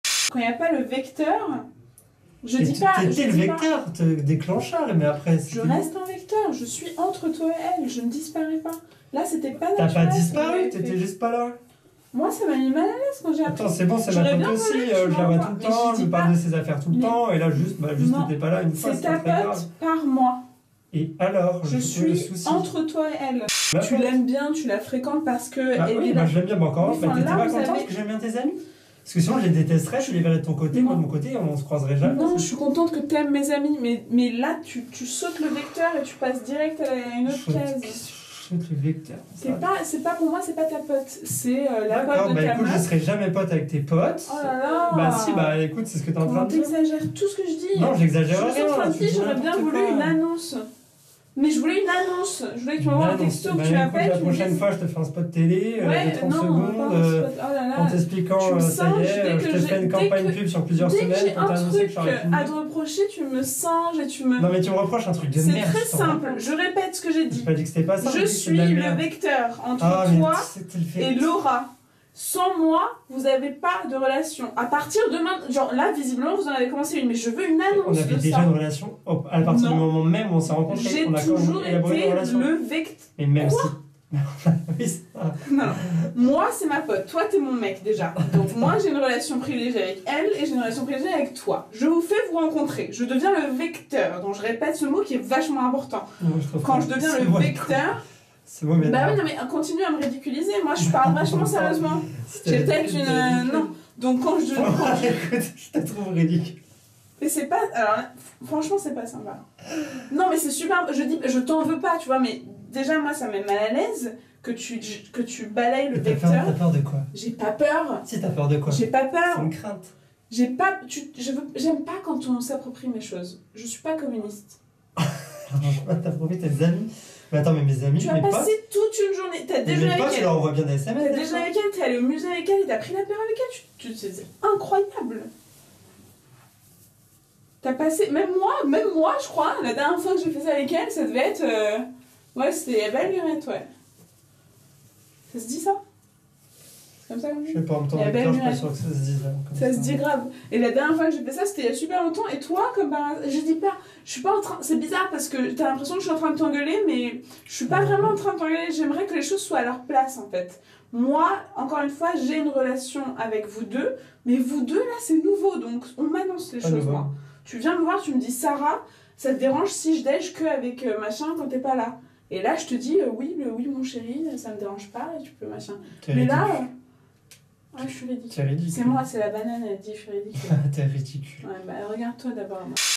LE VECTEUR | the kidults
Quatre colocs dans une salle de bain.
Four roommates in a bathroom.